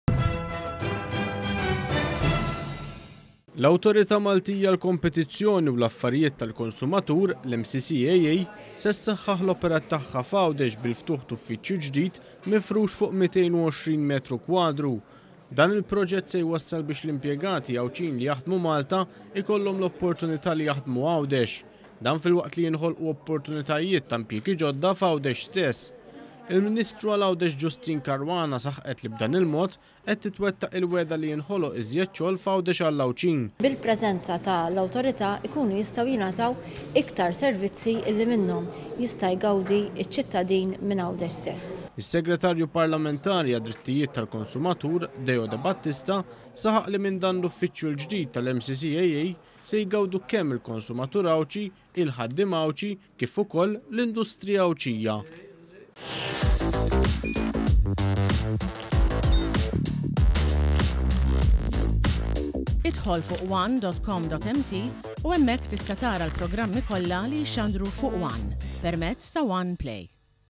L-MCCAA B’UFFIĊĊJU ĠDID F’GĦAWDEX
L-awtorità Maltija għall-Kompetizzjoni u l-Affarijiet tal-Konsumatur fetħet uffiċju ġdid f’Għawdex biex issaħħaħ l-operat tagħha f’din il-gżira. Dan l-uffiċċju għandu l-potenzjal li joħloq iżjed xogħol għall-Għawdxin f’Għawdex stess.